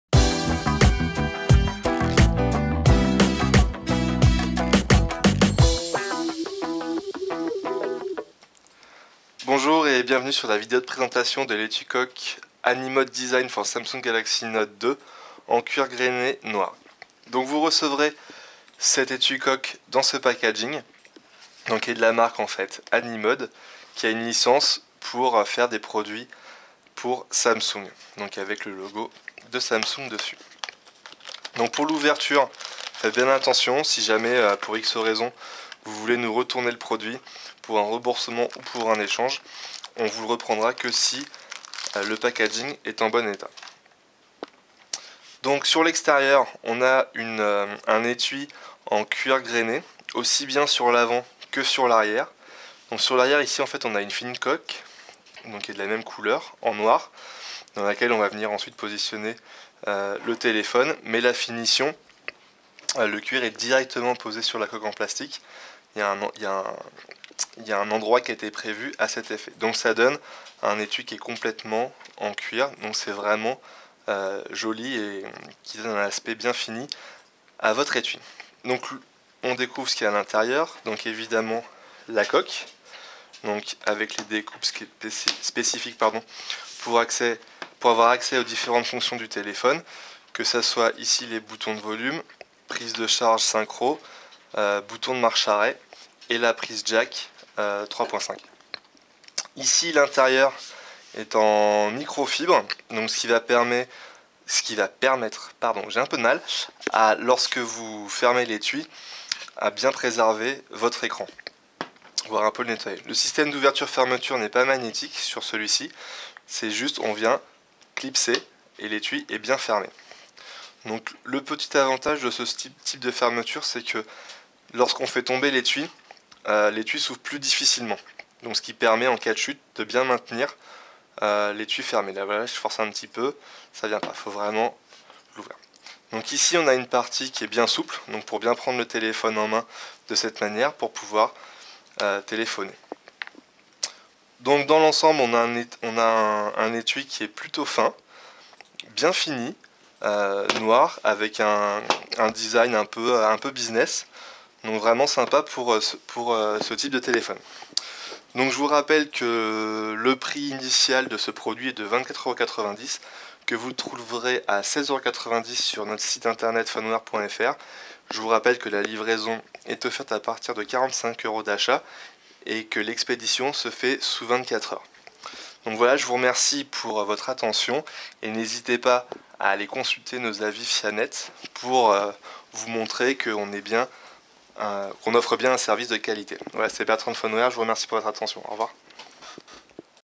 Etui coque Anymode design for Samsung Galaxy Note 2 N7100
Présentation vidéo de l'étui coque Anymode designed for Samsung Note 2.